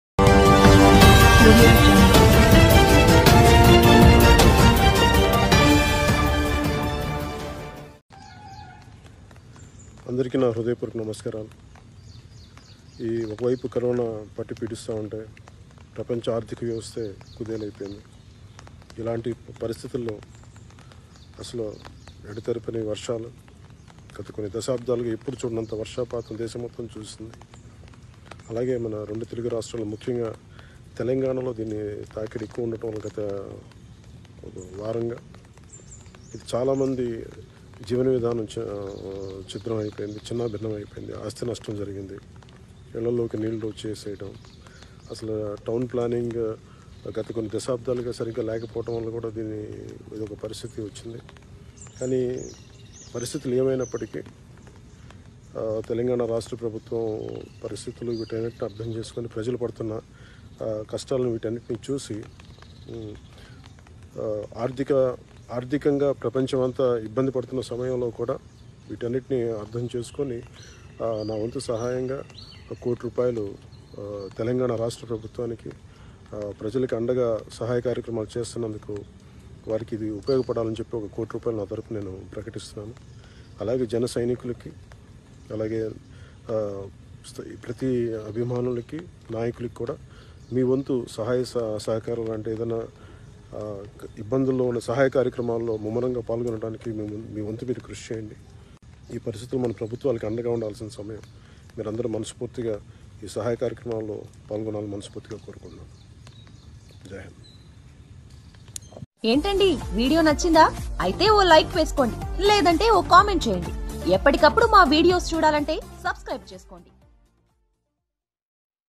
కోటి రూపాయలు విరాళం | Pawan Kalyan Donates One Crore Rupees to CM Relief Fund | Hyderabad Floods
Watch#